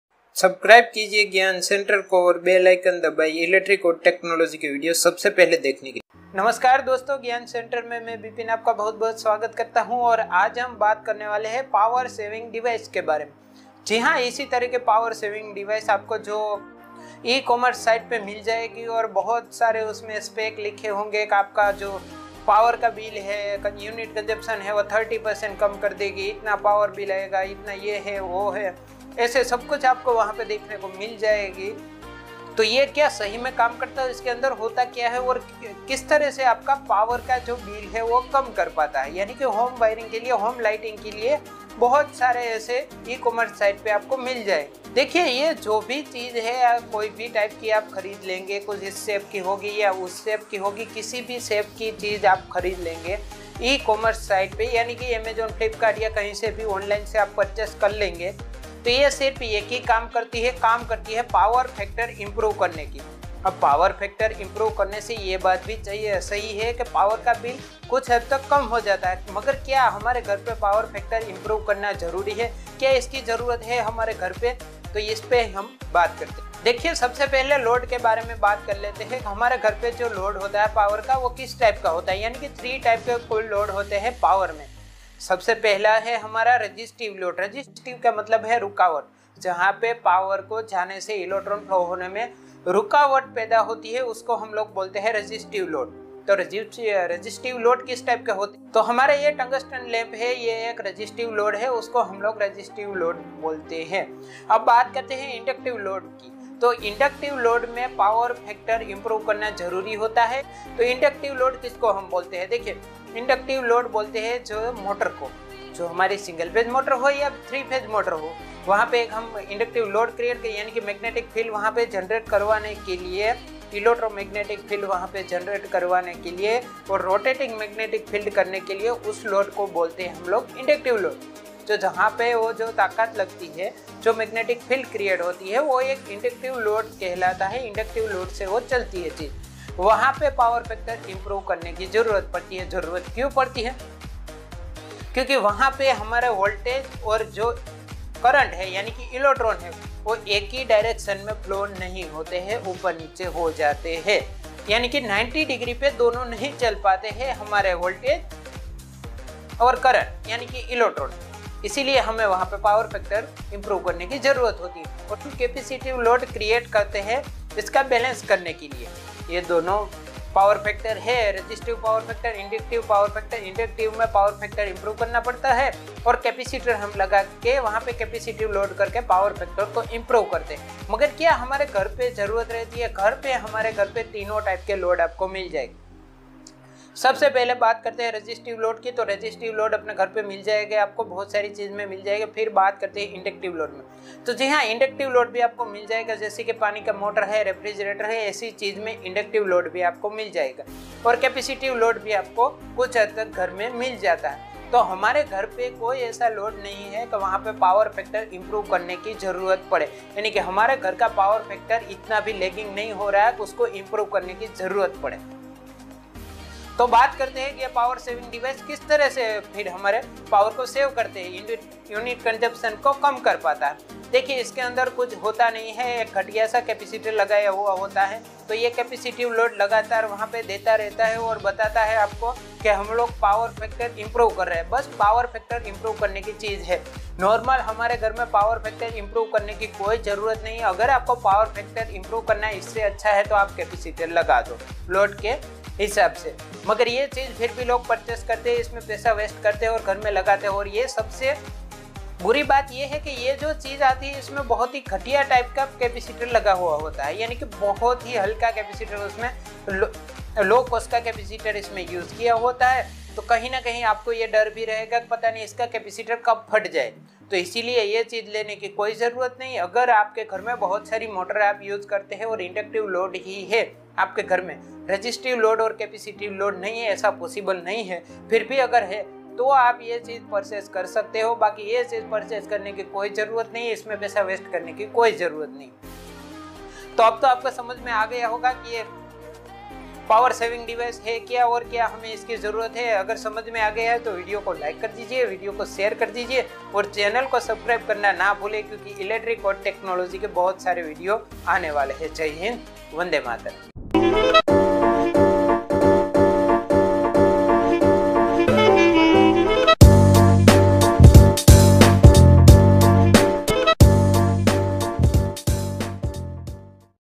POWER SAVING DEVICES REALITY,POWER SAVING DEVICE BIJLI BACHATA HAIY ?
Dosto Maine aapse share kiya haiy ki power saving devices kya haiy kya yeah power saving karta haiy ummid haiy ki Ye video aapko pasand aayega

Dosto Hamare channel ka name change hua haiy technical gyan se Gyan Centre